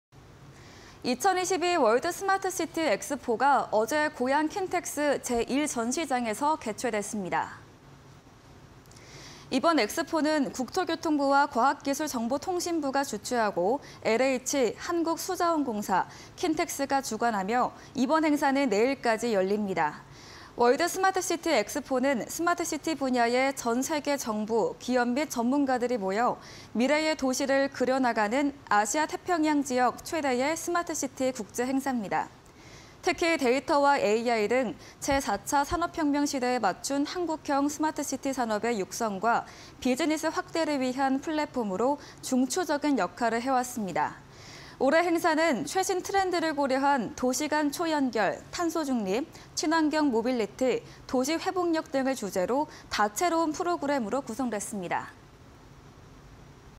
2022 월드 스마트 시티 엑스포 개막... 아시아 최대 규모 (20220901)
‘2022 월드 스마트 시티 엑스포(WSCE 2022)’가 어제 고양 킨텍스 제1전시장에서 개최됐습니다.
이번 엑스포는 국토교통부와 과학기술정보통신부가 주최하고 LH(한국토지주택공사), 한국수자원공사, KINTEX가 주관하며 이번 행사는 내일까지 열립니다.
월드 스마트시티 엑스포는 스마트 시티 분야의 전 세계 정부, 기업 및 전문가들이 모여 미래의 도시를 그려나가는 아시아·태평양 지역 최대의 스마트 시티 국제 행사입니다.
특히, 데이터와 AI 등 제4차 산업혁명 시대에 맞춘 한국형 스마트 시티 산업의 육성과 비즈니스 확대를 위한 플랫폼으로 중추적인 역할을 해왔습니다.
올해 행사는 최신 트렌드를 고려한 도시 간 초연결, 탄소중립, 친환경 모빌리티, 도시 회복력 등을 주제로 다채로운 프로그램으로 구성됐습니다.